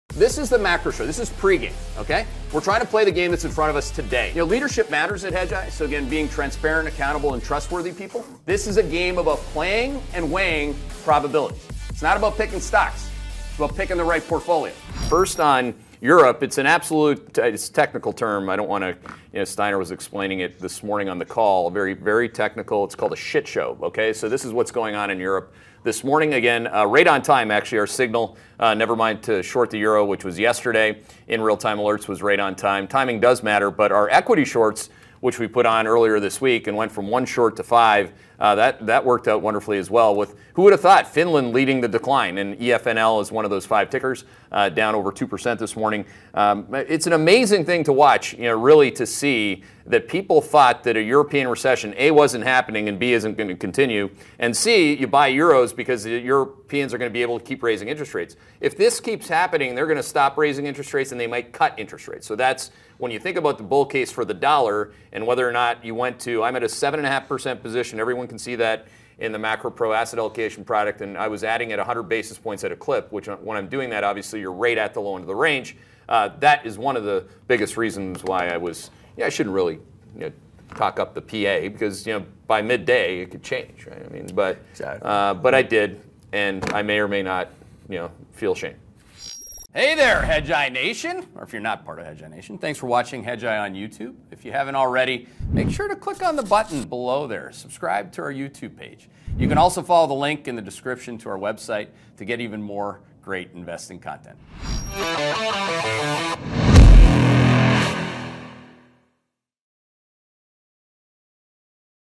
McCullough: Europe Is A Sh*t Show
On The Macro Show this morning, Hedgeye CEO Keith McCullough reviewed the Top-3 things looming on his radar screen. During this excerpt, Keith highlights some of the insanity going on with respect to Europe.

“It’s an amazing thing to see that people thought 1) a European recession wasn’t happening; B) isn’t going to continue; and C) people are buying Euros thinking they’re going to continue raising interest rates,” explains McCullough.

“If this keeps happening, they’re going to stop raising interest rates and they may cut interest rates.”